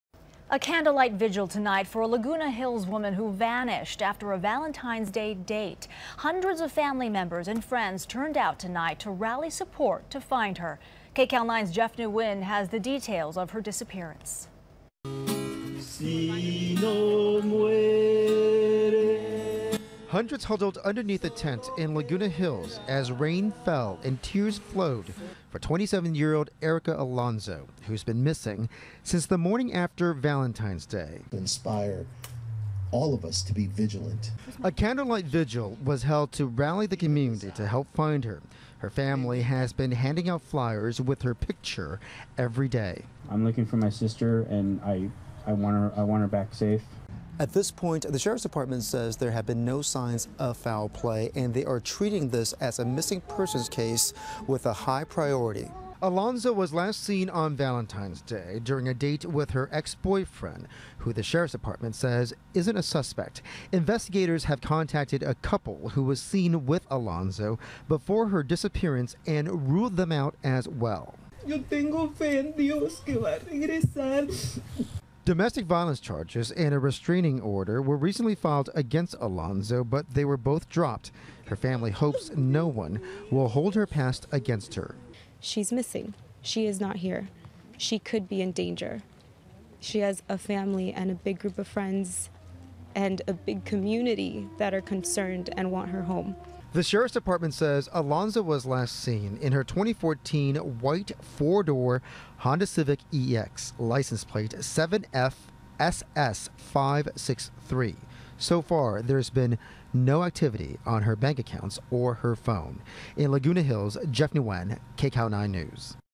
Candlelight Vigil Held For Missing Laguna Hills Woman, 27
Friends and family gathered Saturday evening to hold a vigil honor of 27-year-old Erica Alonso. She was last seen on Feb. 15. Jeff Nguyen reports.